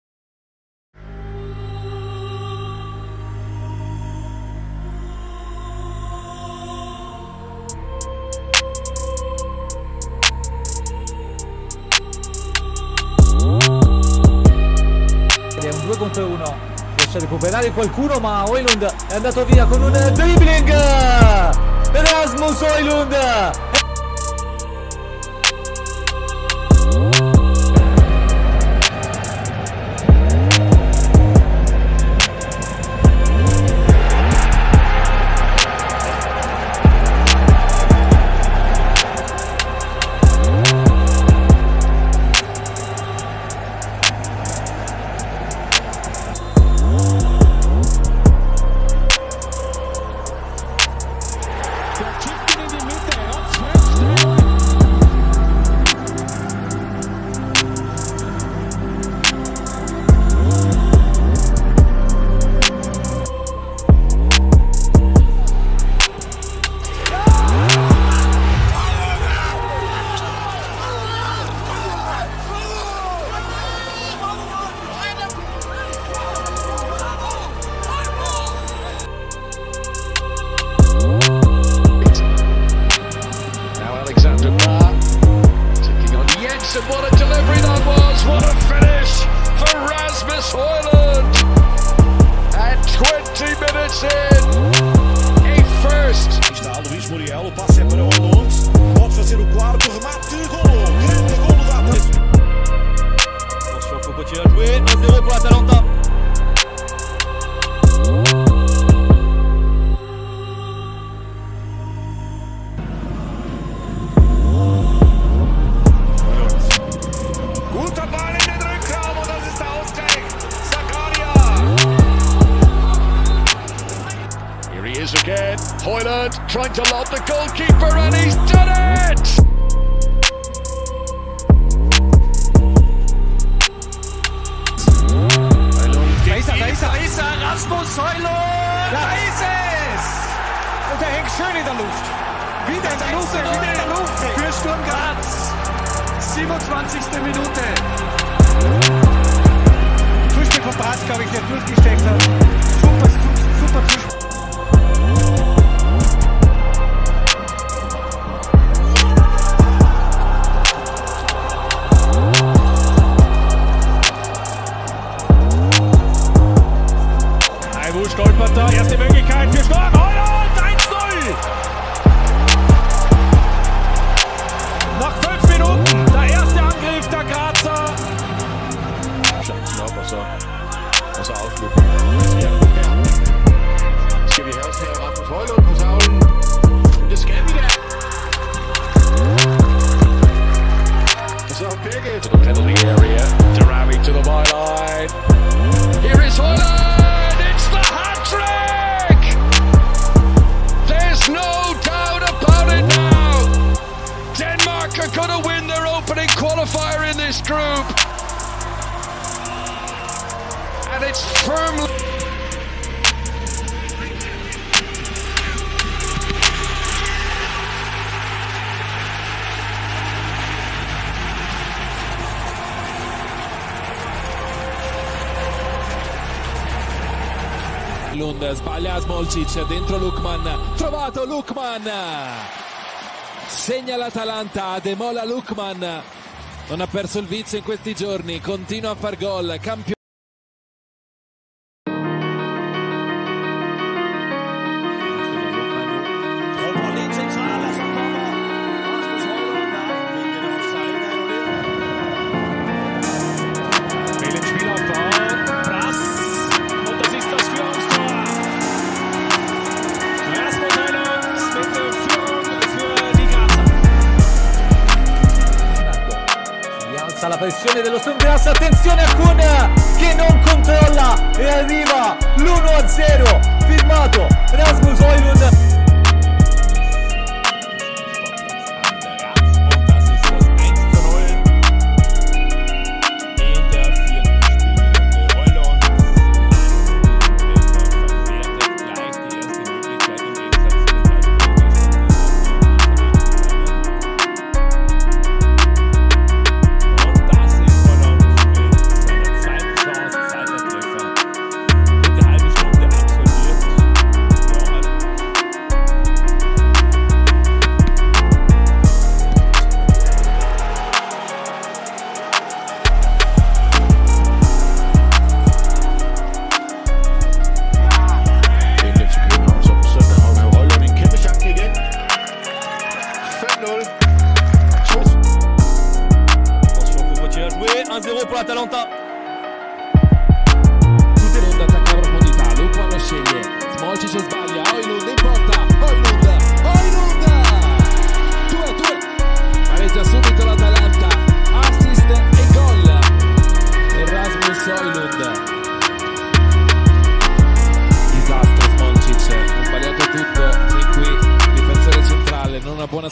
Rasmus Højlund Will Be Manchester United’s Erling Haaland
Today On Goal Hero we're showcasing Rasmus Højlund

Meet Højlund, the talented 20-year-old Danish foward, making waves at Atalanta, with Manchester United's interest!